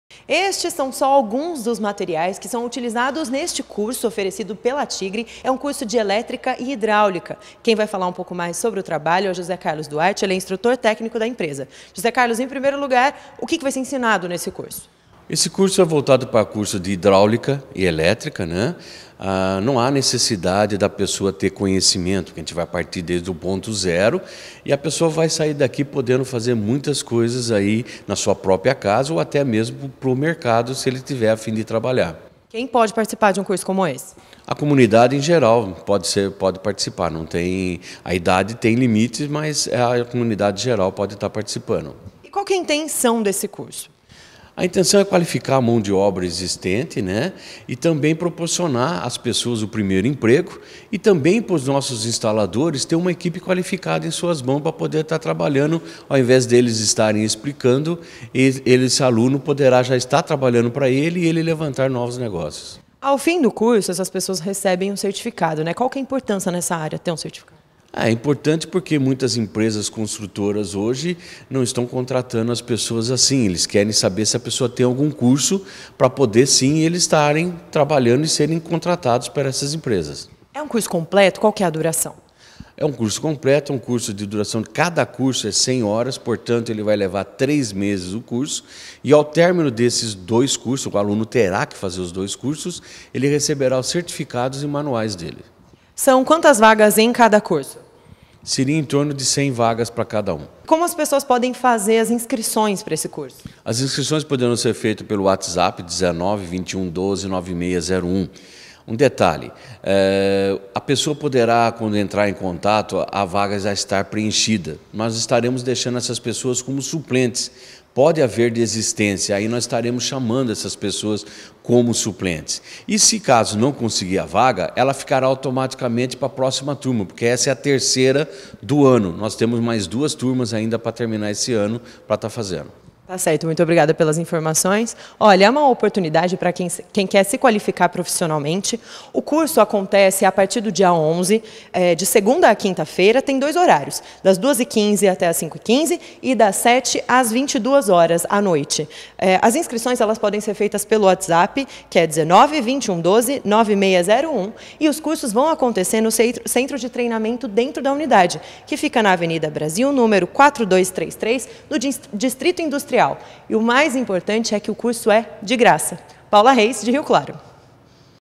Empresa oferece curso gratuito de hidráulica e elétrica
Com duração de cem horas, o curso é voltado para toda a comunidade. Ao final das aulas, o aluno recebe certificação.